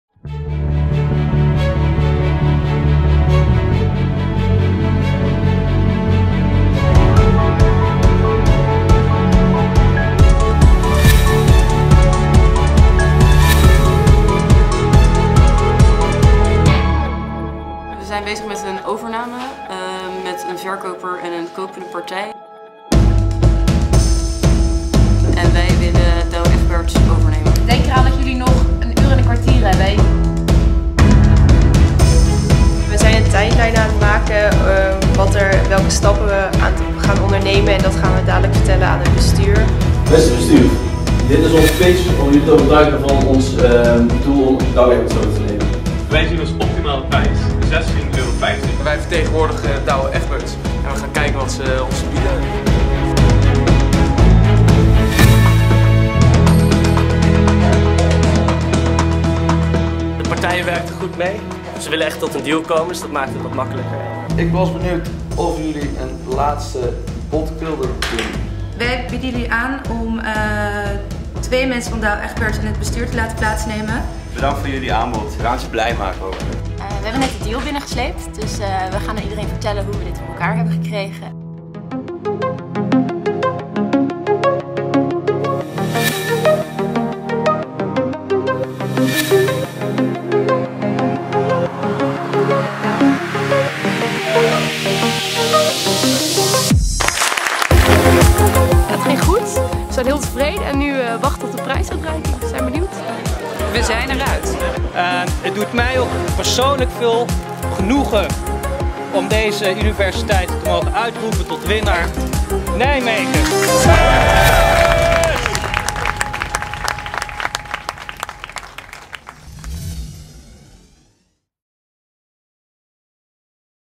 Universiteitsstrijd Rechten 2015: aftermovie finaledag.
Op donderdag 18 juni vond de finale van Universiteitsstrijd Rechten 2015 plaats. Negen universiteitsteams van de negen verschillende Rechtenfaculteiten streden tegen elkaar om de titel.  Speciale dank gaat uit naar NautaDutilh, Allen & Overy, De Brauw Blackstone Westbroek, Linklaters en Stibbe.